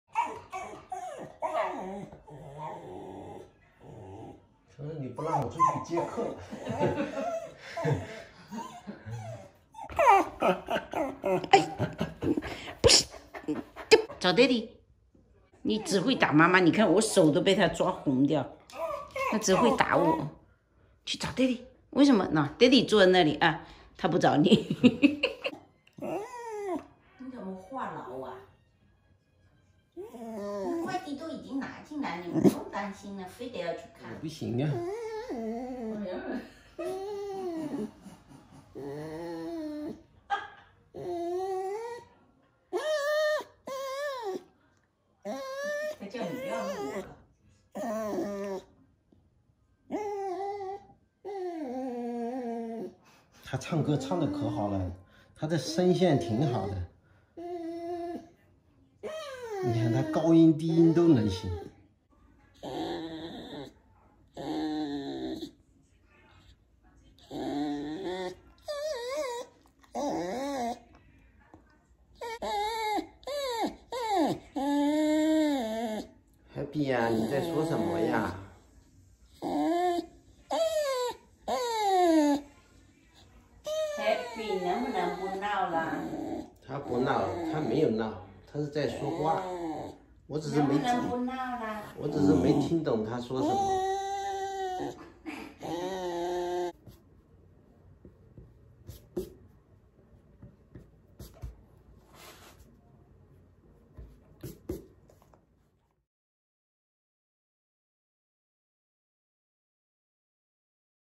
【一分钟短视频】快递来了 没让狗子去接 不停抱怨像话唠
详细视频看： HAPPY 5岁啦！姐姐做蛋糕 皮皮全程观看 爹地妈咪送礼物 最后还有节目表演 【Garden Time 田园生活分享】